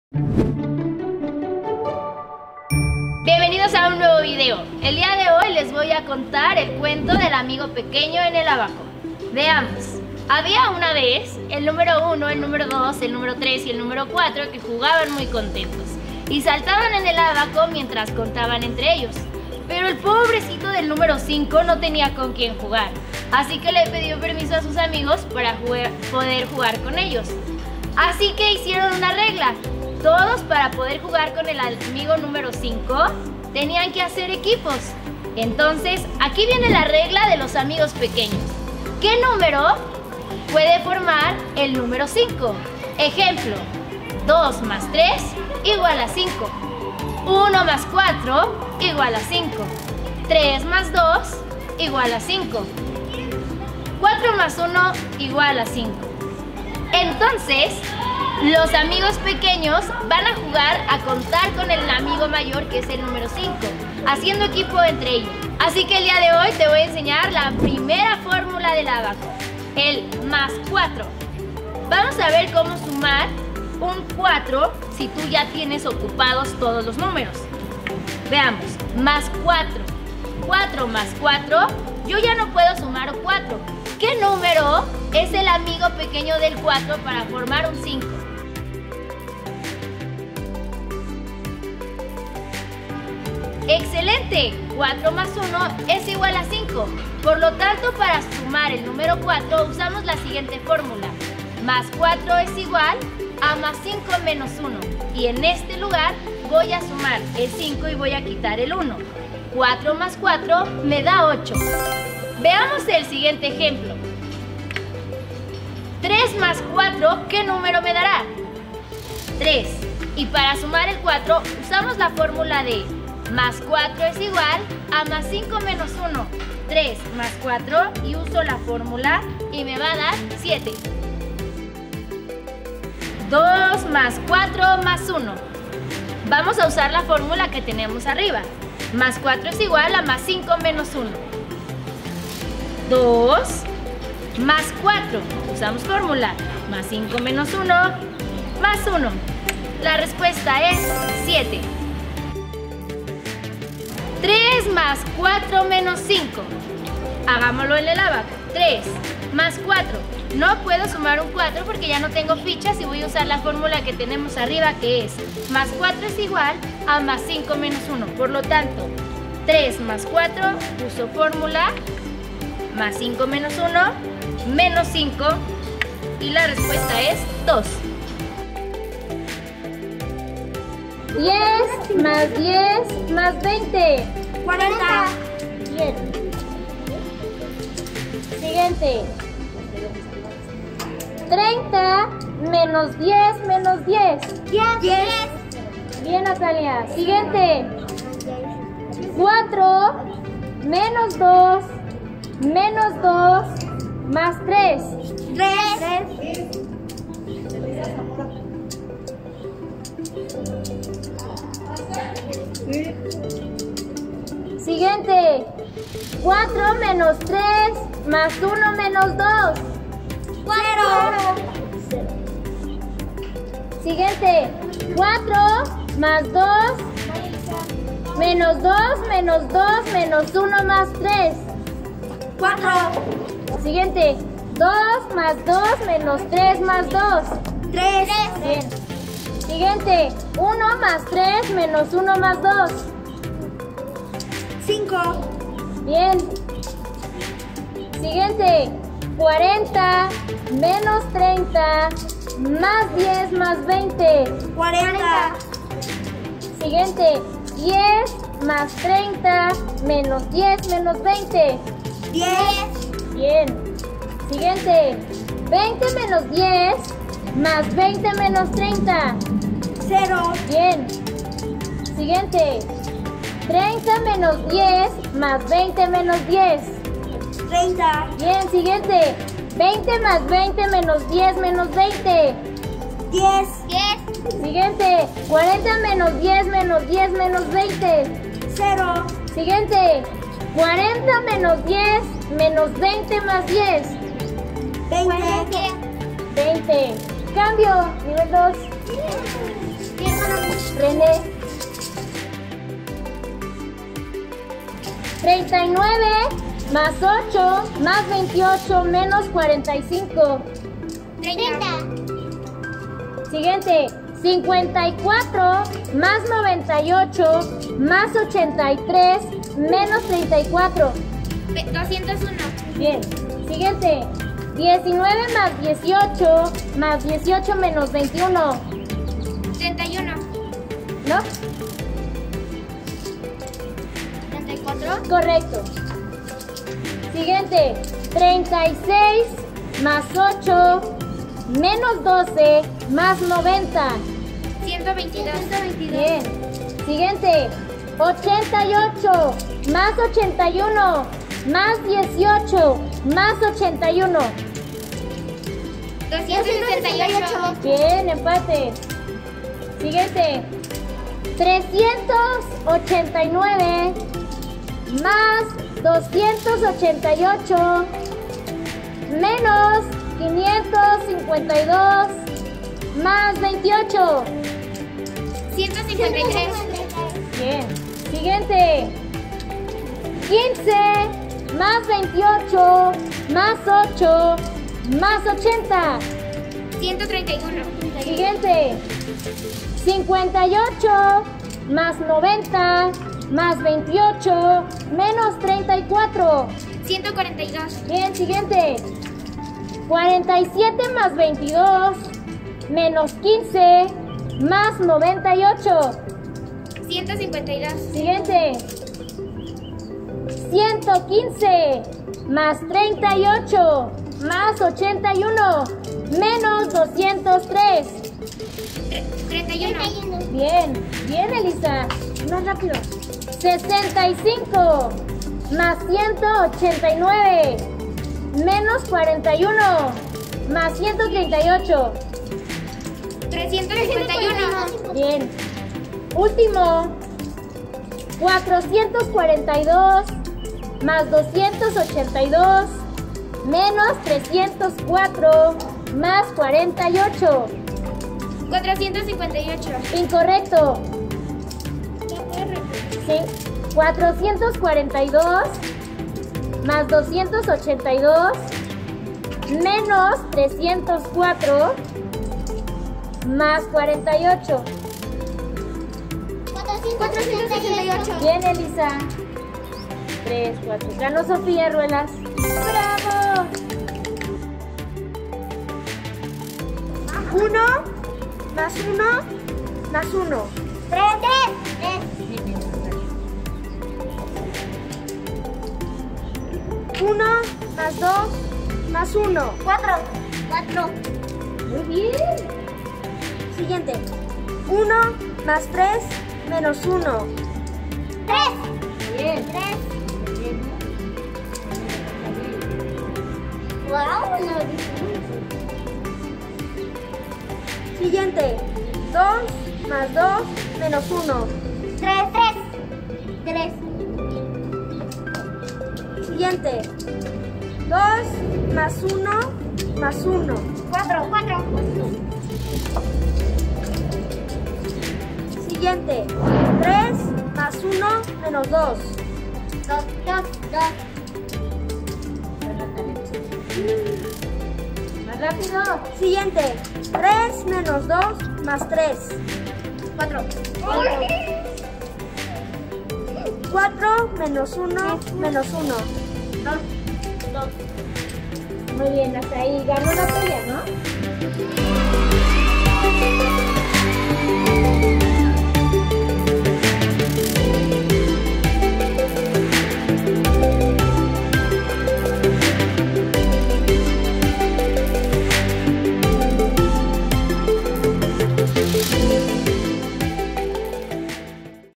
El cuento del amigo pequeño, uso de Soroban. Flash Anzan 3.
El ábaco 🧮  tiene mecanismos de suma. Existen la sumas encillas y las complejas.

Las sumas sencillas son aquellas en las que al sumar cada cifra en su varilla correspondiente da como total al número igual o inferior a 9. La gran ventaja del Soroban es que al anotar un número sobre otro que ya está anotado se realiza la suma por si misma.

Las sumas complejas se realizan usando fórmulas para agregar el número que falta en la varilla para poder agregar el número que deseas sumar.

♟️ Peones Al Ataque ♟️ es una academia deportiva de GimnasiaOlimpica en donde puedes divertirte mientras desarrollas tus habilidades.